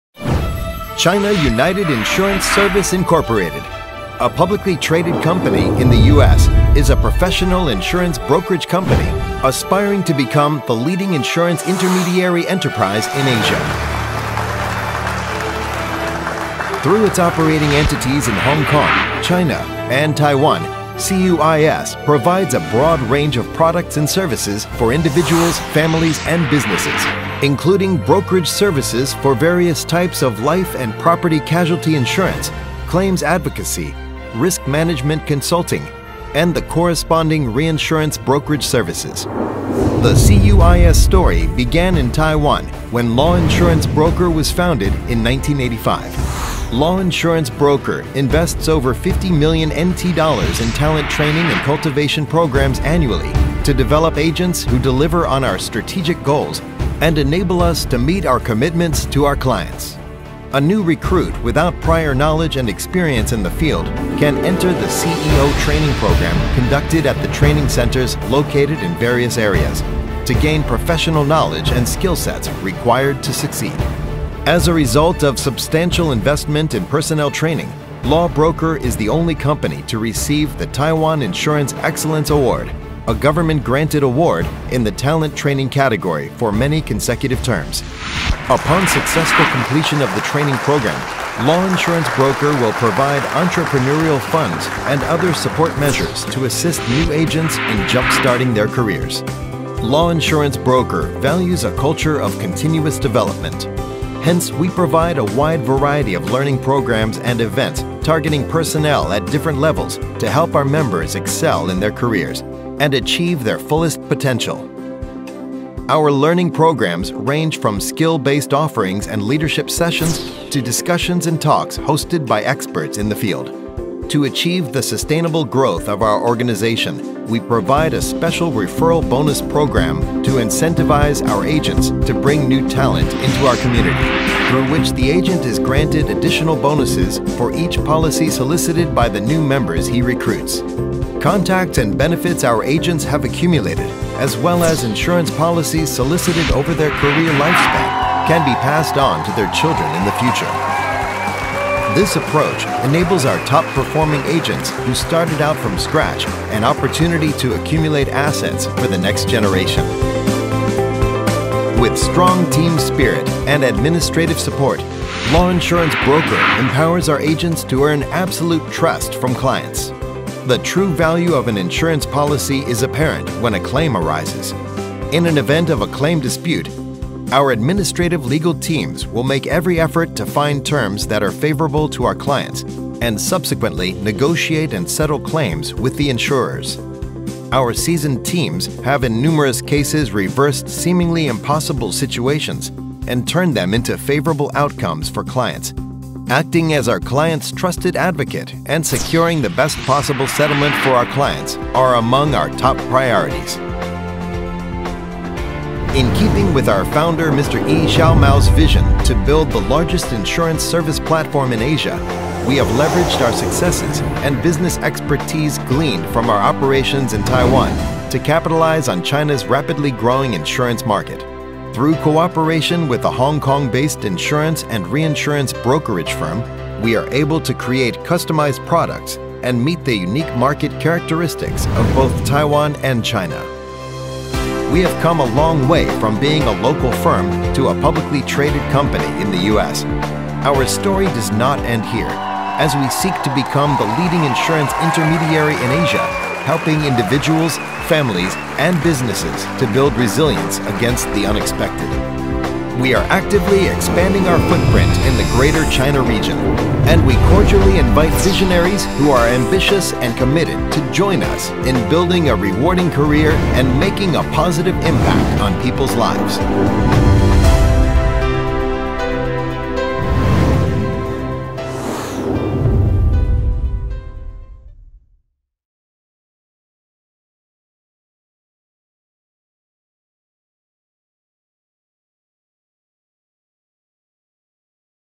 China United Insurance Service, Inc.
With over 30 years of experience in insurance intermediary, China United Insurance Service, Inc. (CUIS) and its subsidiaries provide insurance and reinsurance brokerage services in the Greater China region.